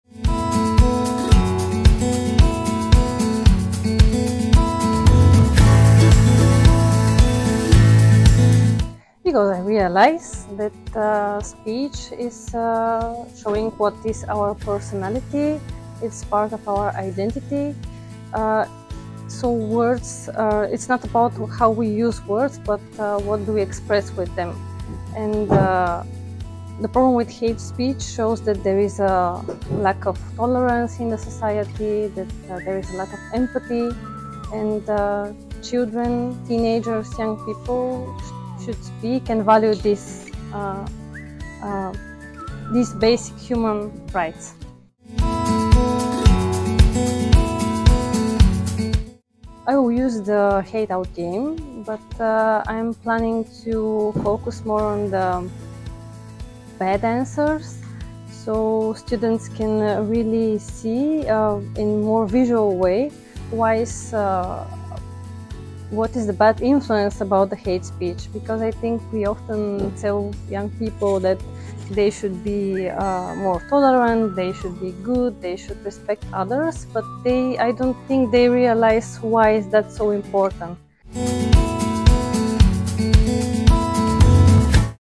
LEAD-Online Ambassadors: Mila Karadakova, teacher, Bulgaria
Join Mila Karadakova, a Bulgarian teacher and passionate LEAD-Online Ambassador, in this video as she shares her experience on combating online hate speech and insights on using the LEAD-Online project's toolkit.

The LEAD-Online project is co-funded by the European Union.